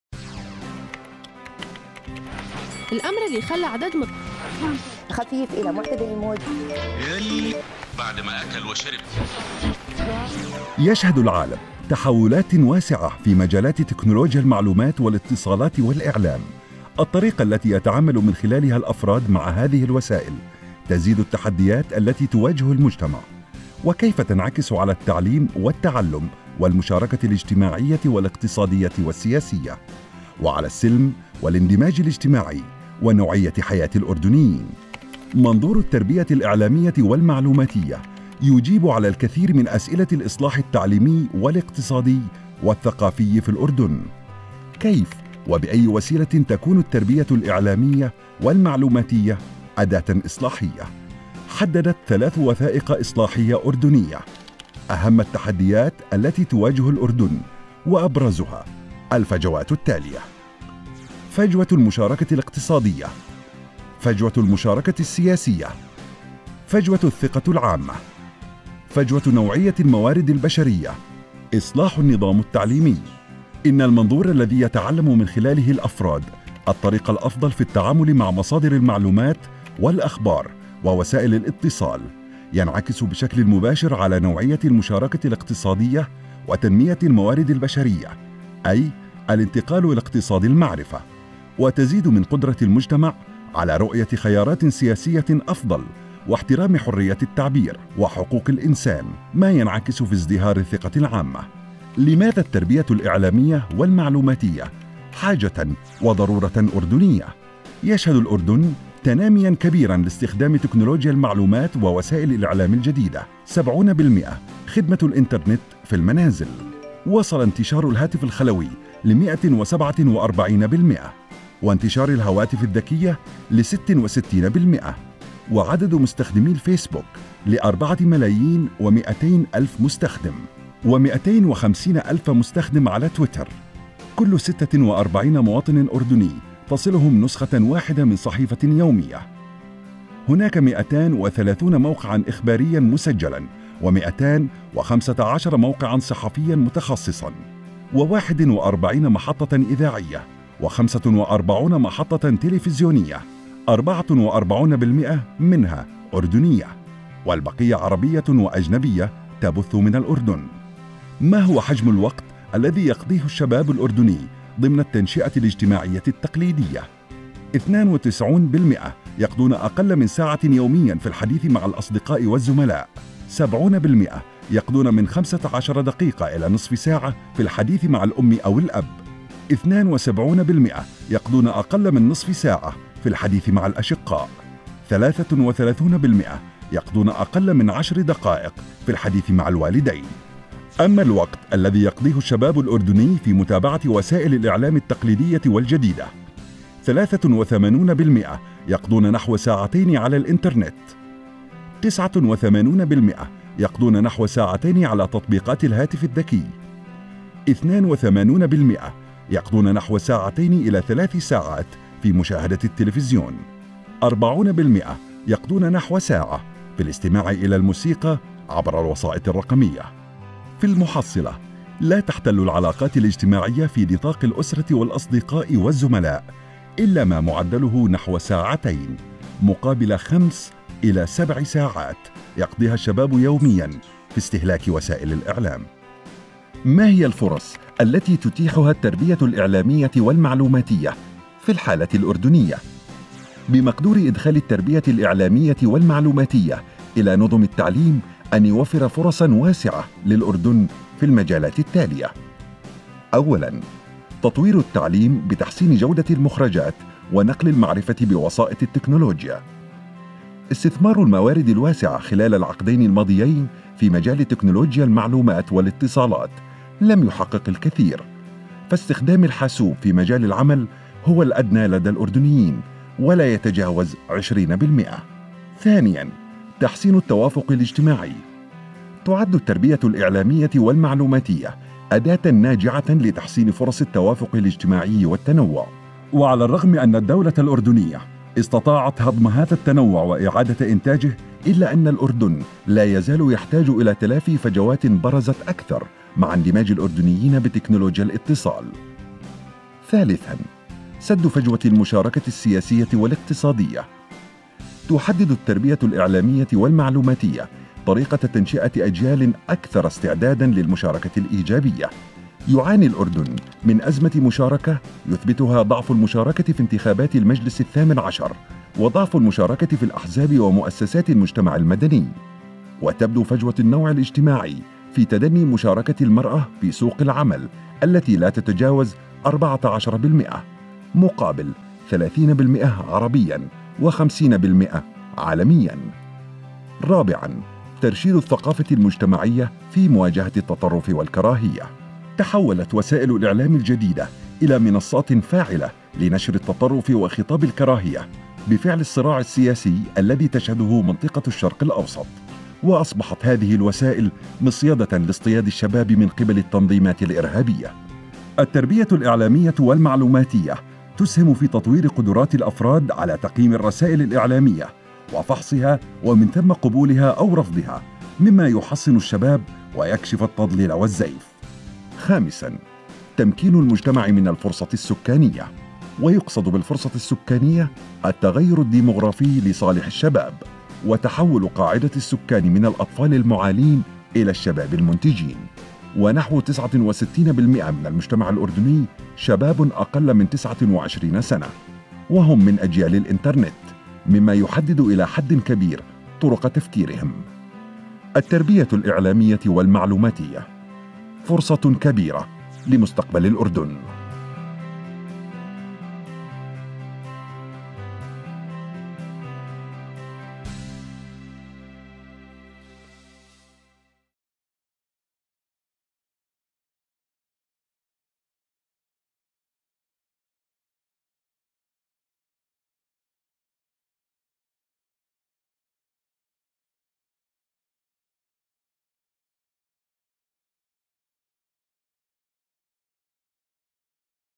Media and Information Literacy in Jordan التربية الاعلامية والمعلوماتية في الاردن
التربية الاعلامية والمعلوماتية في الاردن - الحاجات والفرص
As part of the EU funded and UNESCO Amman Office implemented "Support to Media in Jordan" Project, our project partner Jordan Media Institute (JMI) produced an informative video about the importance of media and information literacy in Jordan.